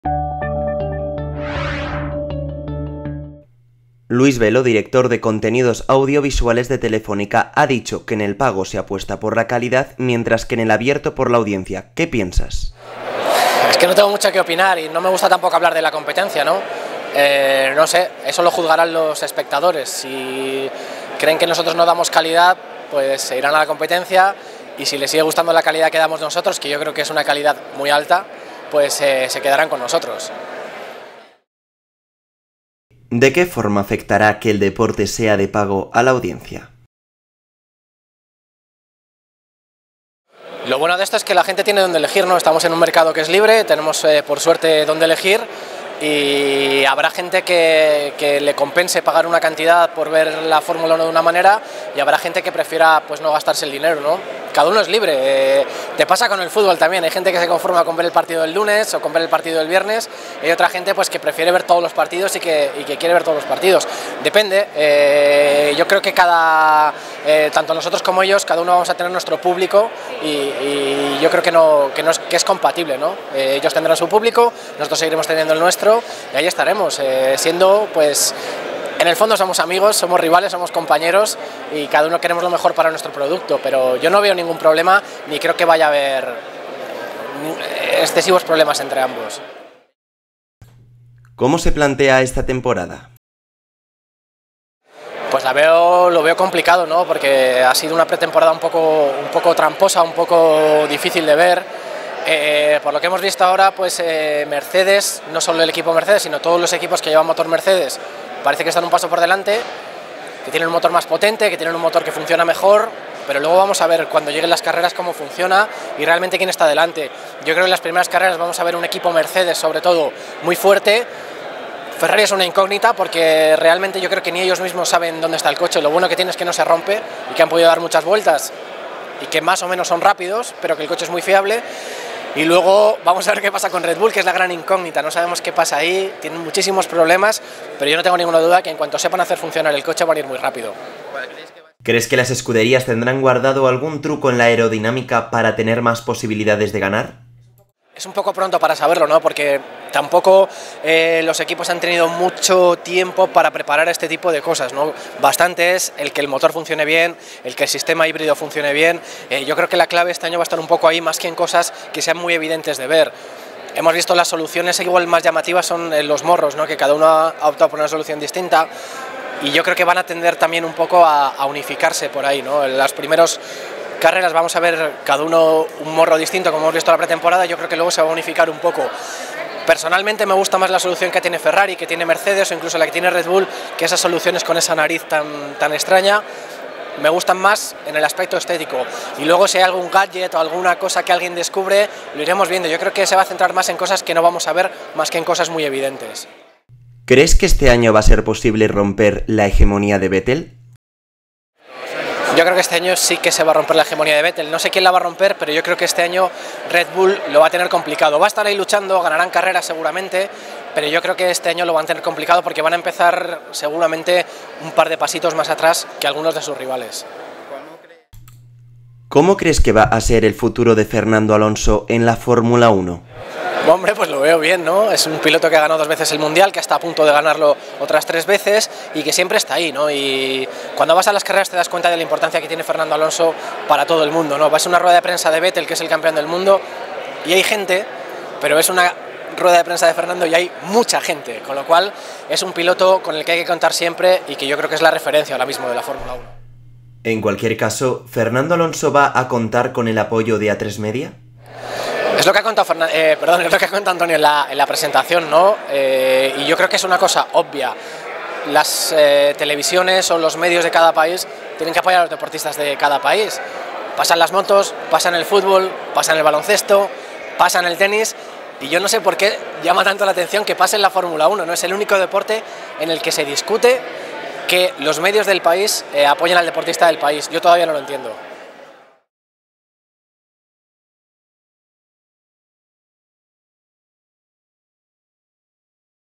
Jacobo Vega: "La Fórmula 1 es el único deporte en el que se discute el apoyo al deportista del país"
Jacobo Vega acompañará a Antonio Lobato durante la narración de las competiciones de Fórmula 1 en Atresmedia.